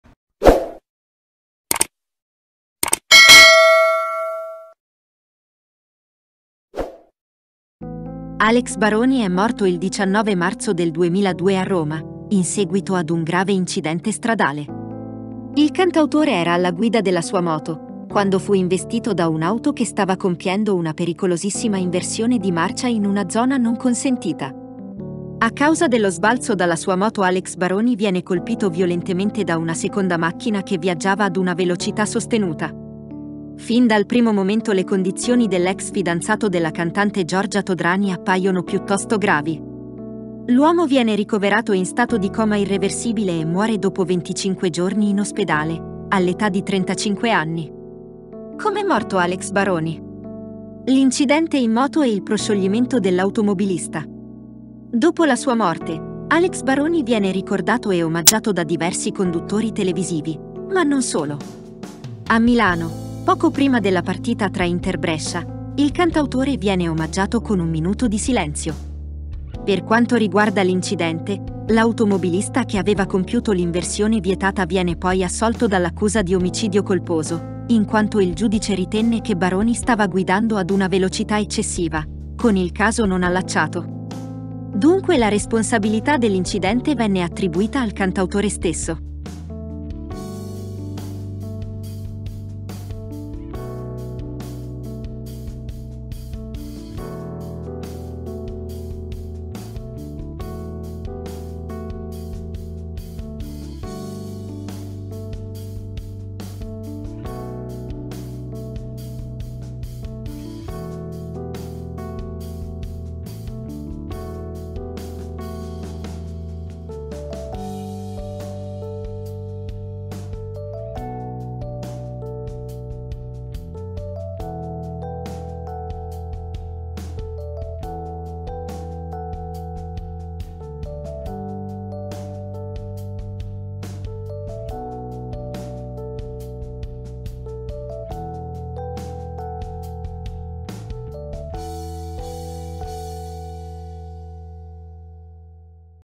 Alex Baroni, com’è morto: l’incidente e l’ex compagna Giorgia Todrani/ Il coma, poi..
Alex Baroni, com’è morto: l’incidente e l’ex compagna Giorgia Todrani. Il coma per venticinque giorni, poi il decesso. E in tv..